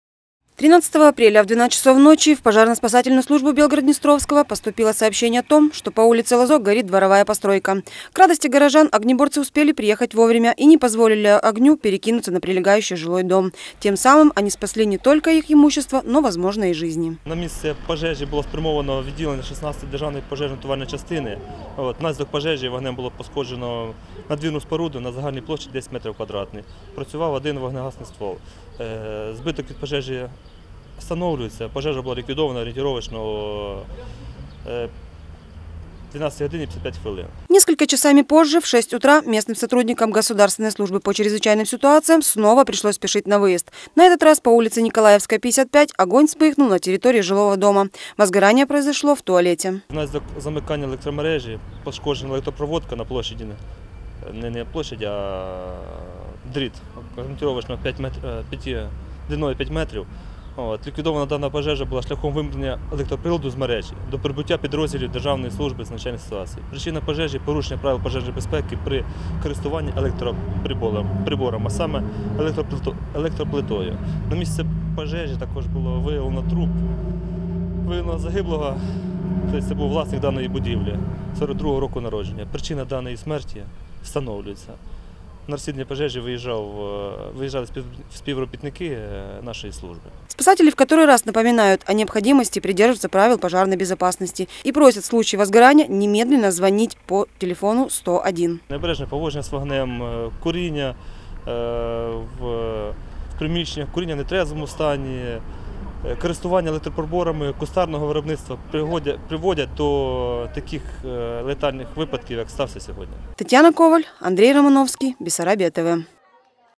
Жаркая ночь
В ніч  на 13 квітня вогнеборцям Аккерману  було спекотно.  На території Білгород-Дністровського сталося кілька пожеж. На превеликий жаль без жертв не обійшлося. Розлогіше дивитеся далі